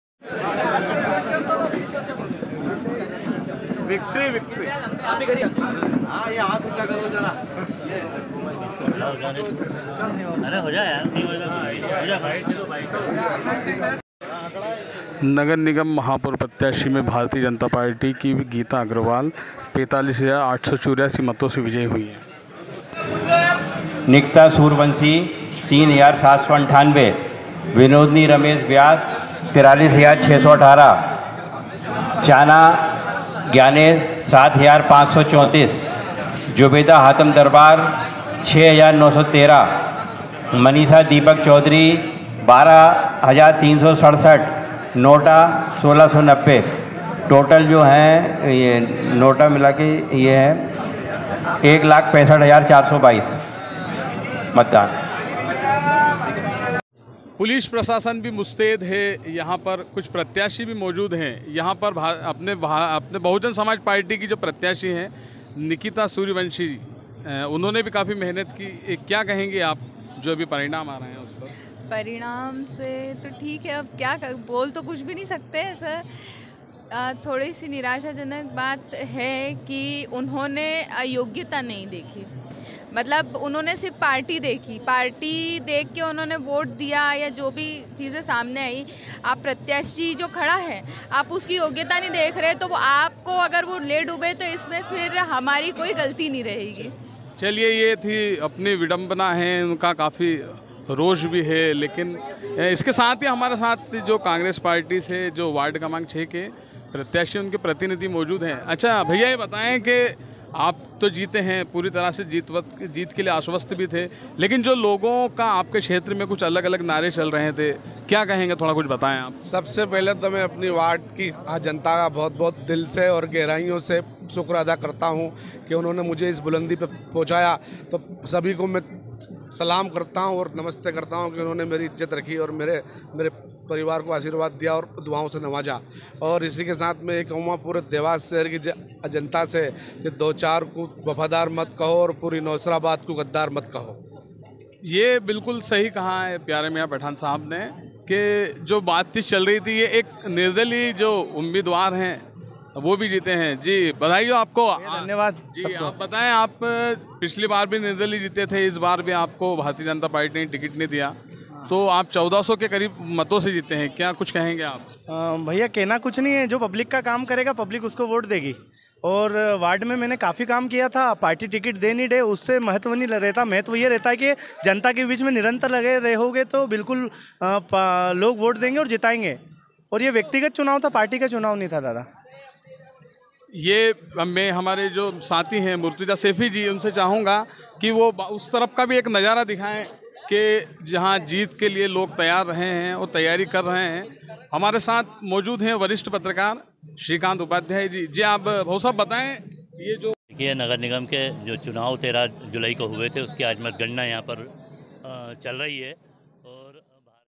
dewas-बीजेपी से गीता अग्रवाल विजय, महापौर में किस प्रत्याशी को कितने वोट, देखिए पूरी जानकारी
20 जुलाई 2022